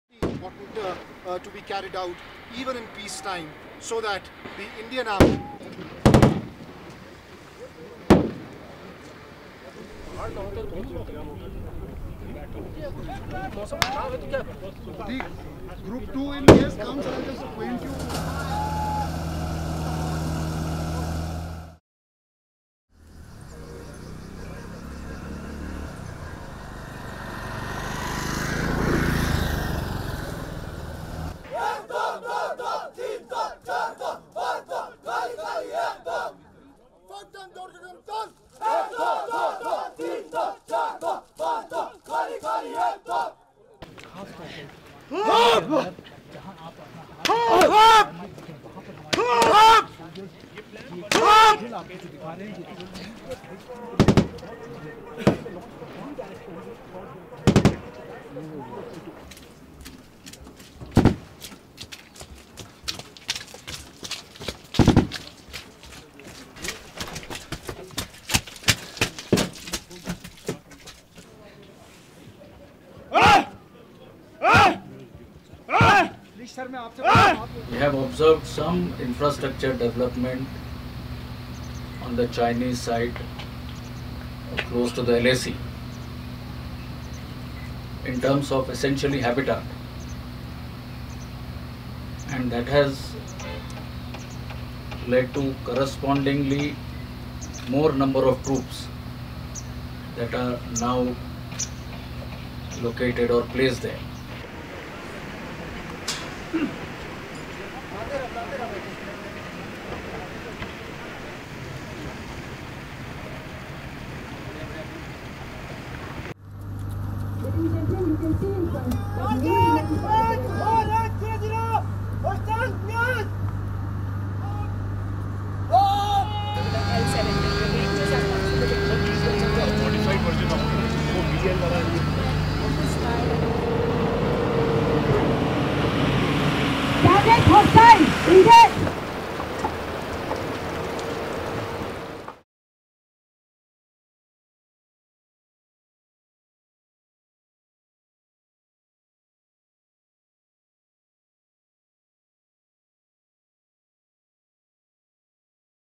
Face à la Chine, l'Inde renforce sa défense le long de sa frontière himalayenne
Un an après des affrontements dans la région du Ladakh qui avaient fait au moins 20 morts parmi les soldats indiens et quatre côté chinois, l'Inde renforce sa défense le long de la frontière qui sépare l'Arunachal Pradesh du Tibet, une zone sous haute tension avec la Chine.

L'Etat indien de l'Arunachal Pradesh, dont Pékin revendique la souveraineté considérant qu'il s'agit du territoire Sud du Tibet, avait été brièvement occupé par la Chine, trois ans après la fuite en mars 1959 du 14e Dalaï Lama, lors d'un conflit bref mais sanglant. (Avec AFP)